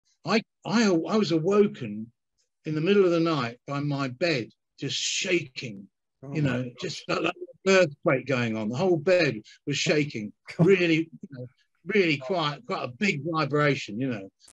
Nub TV shorts - actor John Altman shares his ghostly experience
legendary actor John Altman who is known for playing Nick Cotton in BBC's Eastenders talks to Nub TV about his ghostly experience!